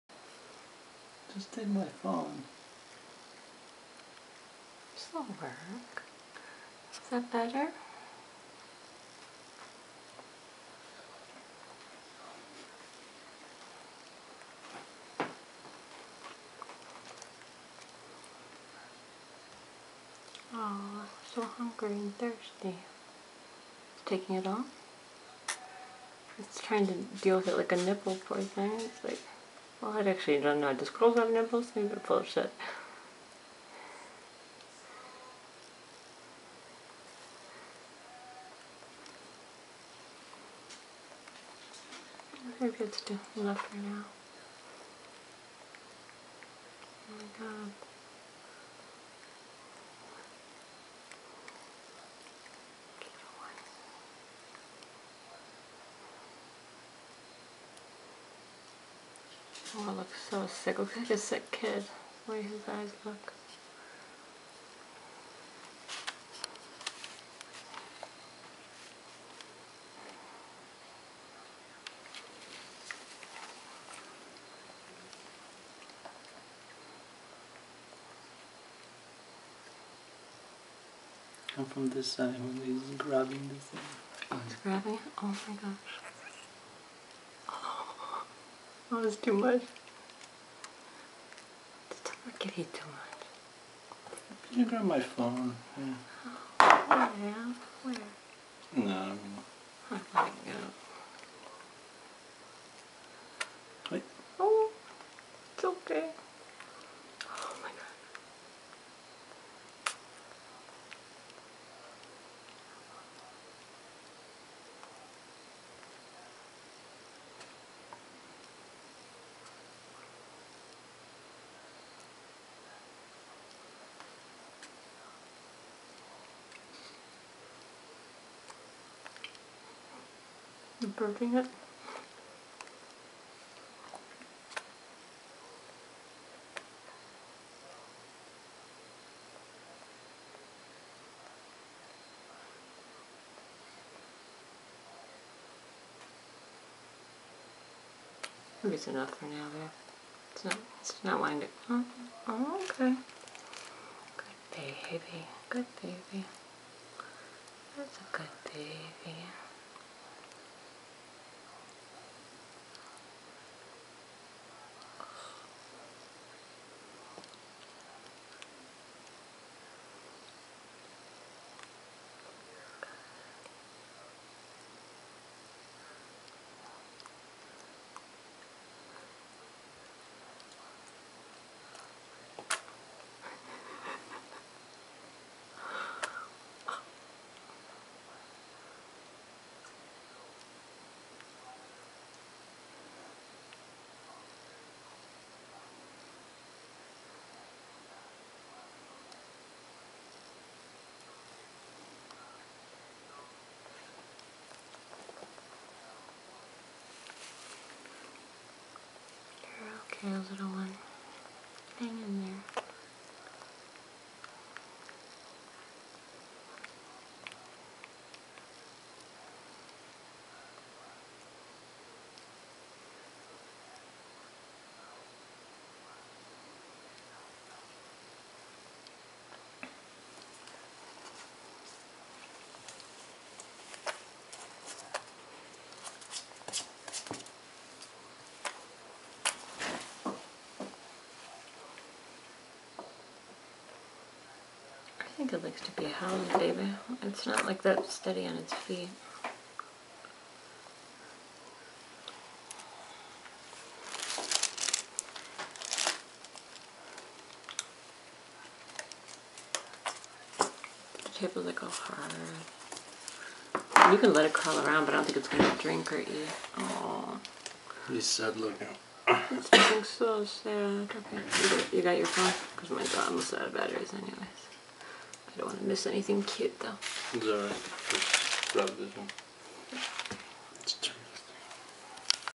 squirrel baby rescue
~ 5 week old grey squirrel that seemed to have been left out in the elements for 12+ hours on a chilly day after phone line workers knocked him out of his nest. although dehydrated and freezing here, he perked right up after time cuddled in an electric blanket and some sugar water/pedialyte. we dropped him off safe and sound to the nearest wildlife rescue first thing the next morning.